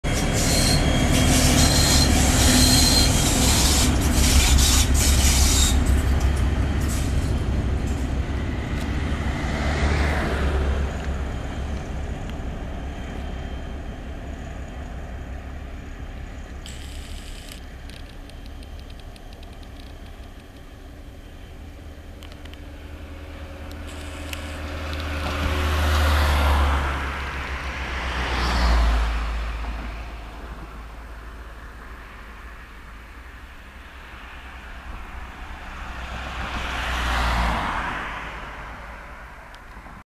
Operacja zarżnięte suki, cz. II
Ciąg dalszy konwoju lokomotyw z Zamościa do Lublina w dniu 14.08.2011 r. W Zwadzie walentyna przez kwadrans nie mogła wystartować, ale w końcu jakoś się jej udało. Co ciekawe jakąś godzinę później do Zamościa z Chełma przyjechała inna SM48 po zepsuta 120 i zabrała ją do szopy w Chełmie.